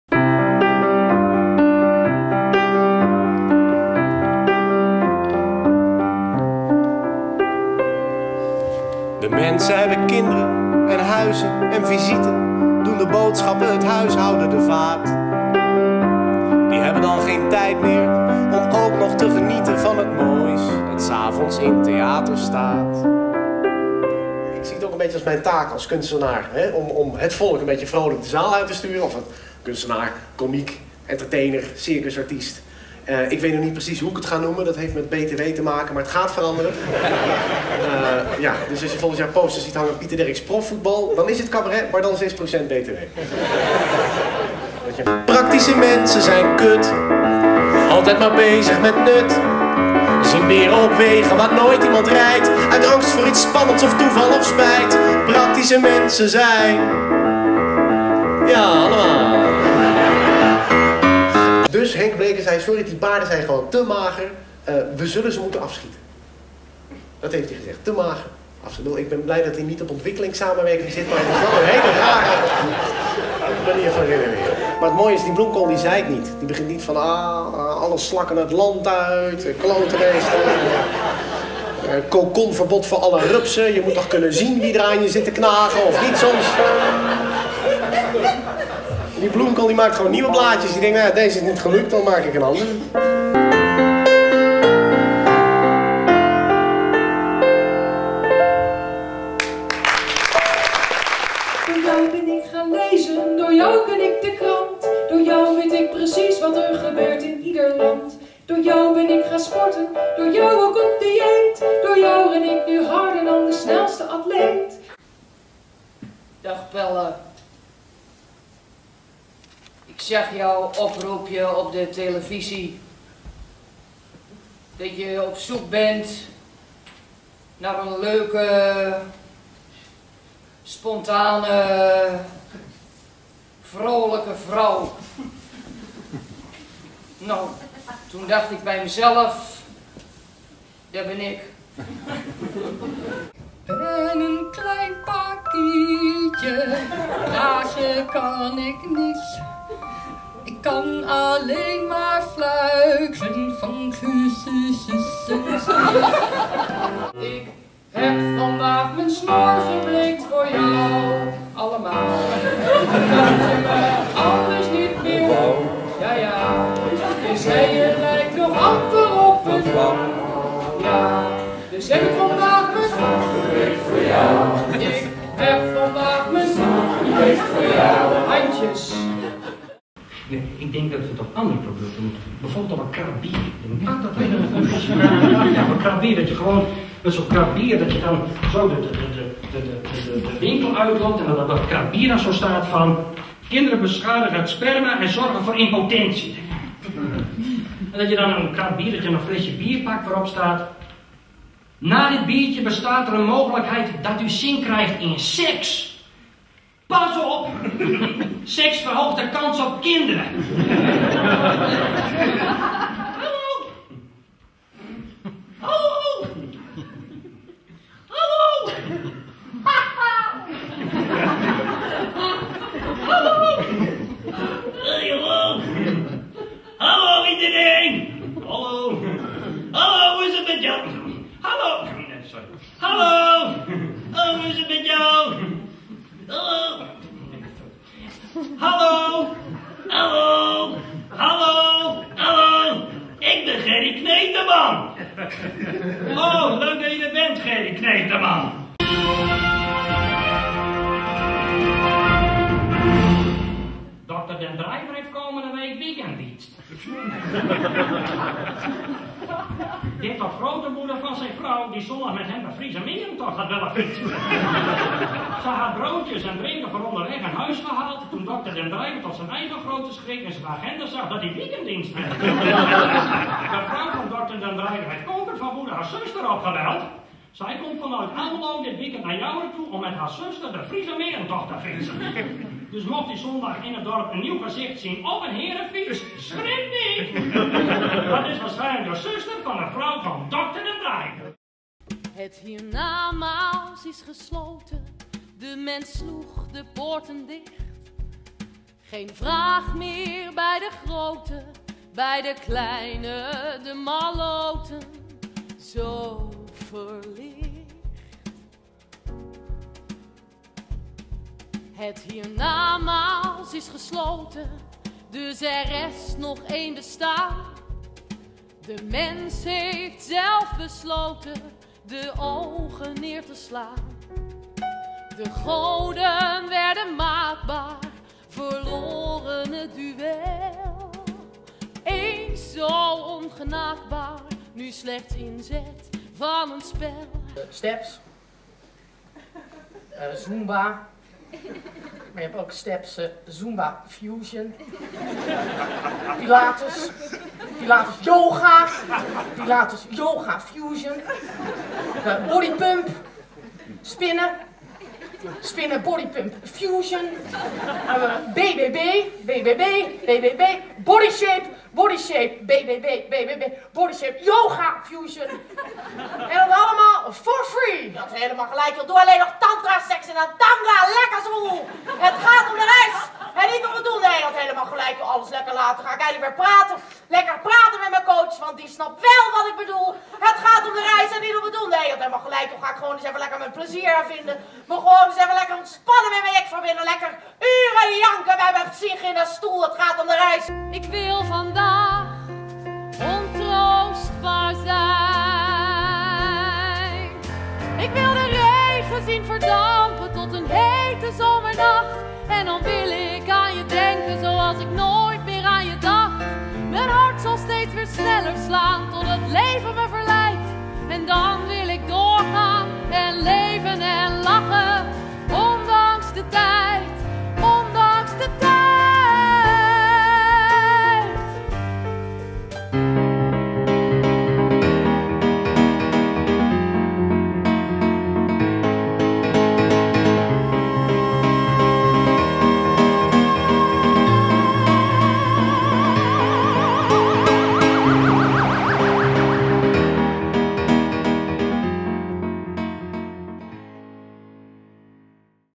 Hummelinck_Cabaret_V2_01_H264.mov
Hummelinck Stuurman presenteert: Jong cabarettalent: Pieter Derks, Yora Rienstra, Ronald Smink en Louise Korthals. Voor boekingen: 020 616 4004.